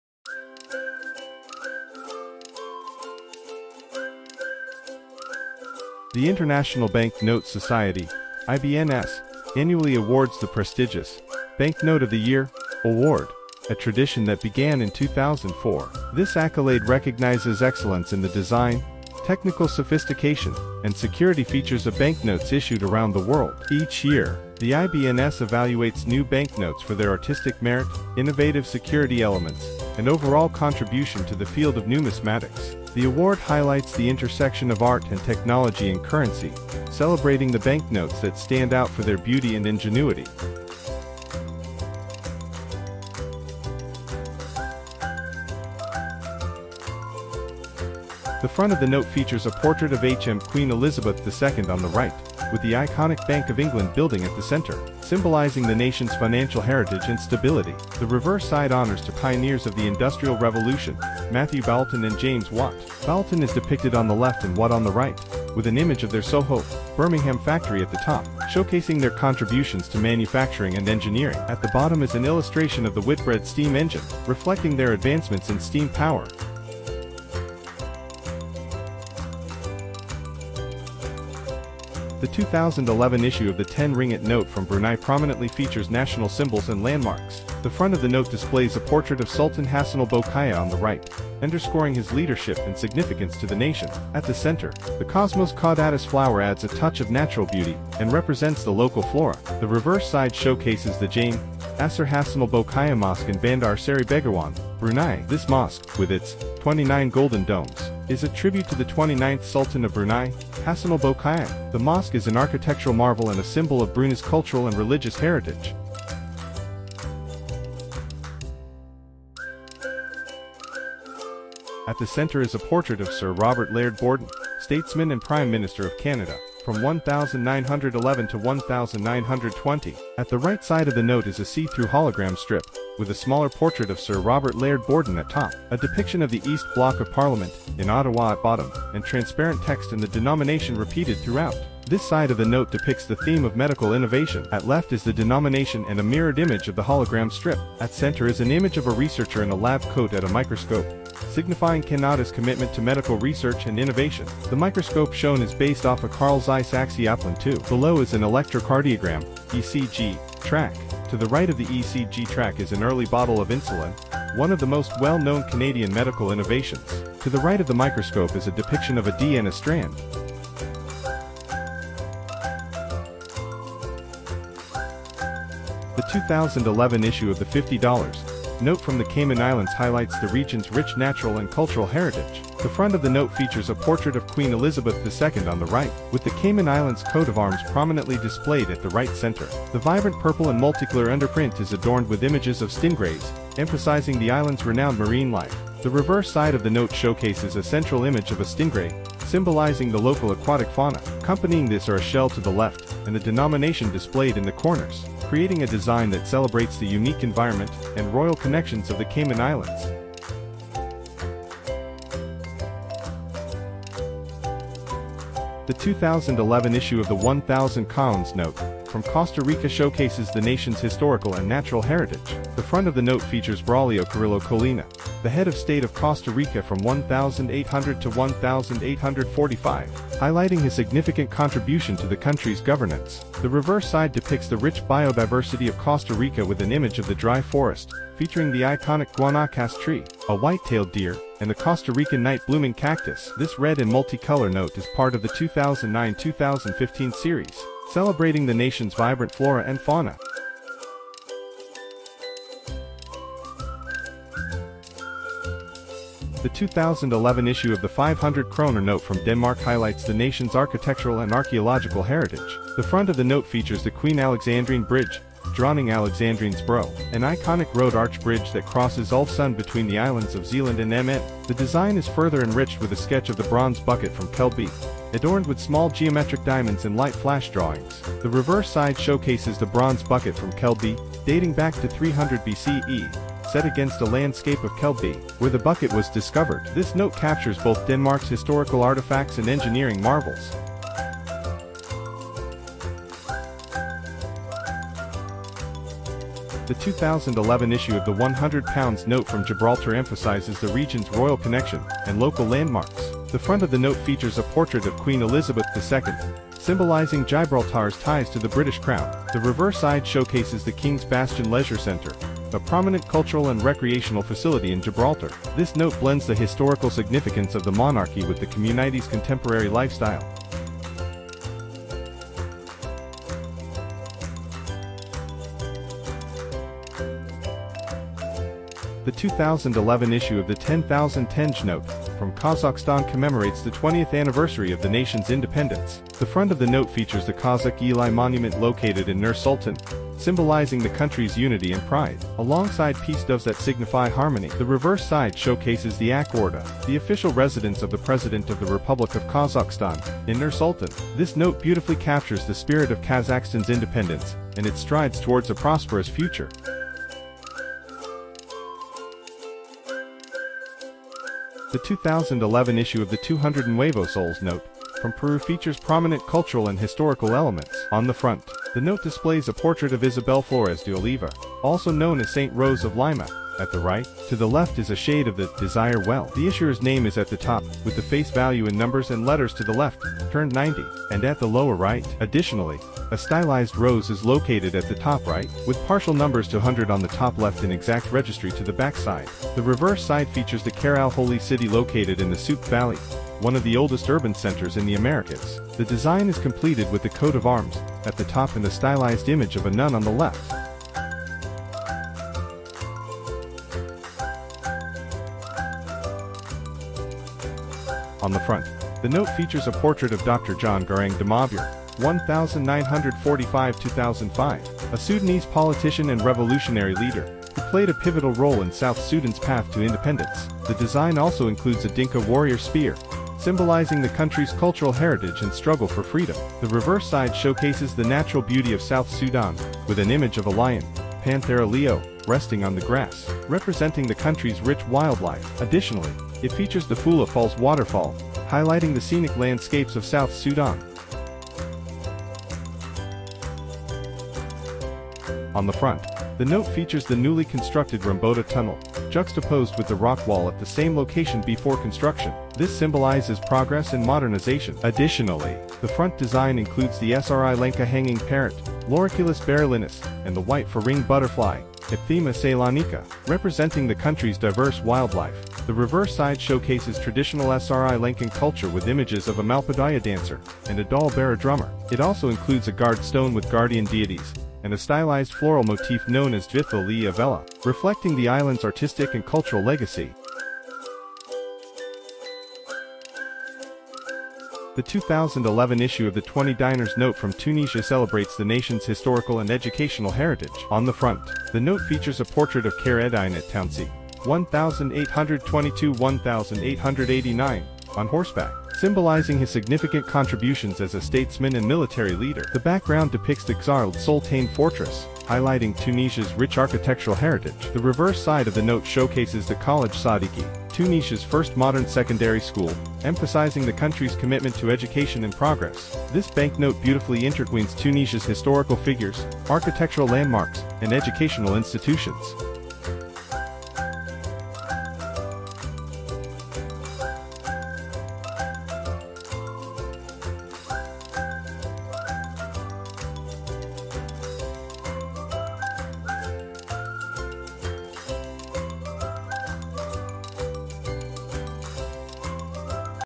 IBNS Banknote of the Year (2011) | Which banknote have won the title of Banknote of the Year?
IBNS Banknote of the Year
The IBNS Banknote of the Year award is an initiative of the IBNS to recognize an exceptional banknote issued each year.
Banknotes nominated must have been issued to the public (specimens and non-circulating currencies are inelligble) for the first time during the year of the award, and must have artistic merit and/or innovative security features, and be in general circulation.
Once nominations have closed, the winner will be decided by a vote of the IBNS, who will consider the artistic merit, design, use of colour, contrast, balance, and security features of each nomination.  The winner will be announced at the the first IBNS Board meeting of the year.

Banknote of 2011 - Nominations

Bank of England 50 Pound Note
Brunei 10 Dollar (Ringgit) Note
Cayman Islands 50 Dollar Note
Canada 100 Dollar Note
Costa Rica's 1,000 Colones Note
Denmark 500 Kroner Note
Gibraltar's 100 Pound note
Kazakhstan 10,000 Tenge Note
Peru 200 Nuevo Soles Note
South Sudan 100 Pound Note
Sri Lanka Rupees 1000 Note
Tunisia 20 Dinar Note

👉Currency of the dependencies, provinces, islands, territories, disputed territories, regions, non recognized by UN, self proclaimed ^^